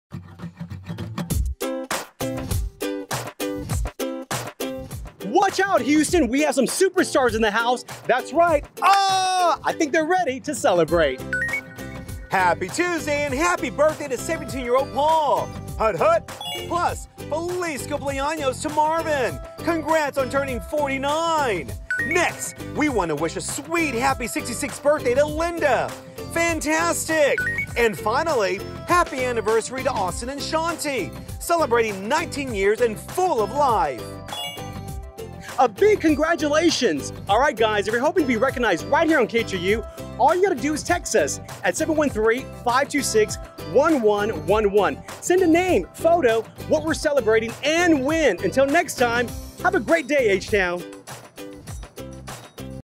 Celebrate! These are your birthdays and anniversary photos shown on Tuesday, October 27, 2020
Thanks for letting us celebrate your big day with you. If you want to recognize a big birthday or anniversary, text a photo and information to us at and we'll try to get it on air!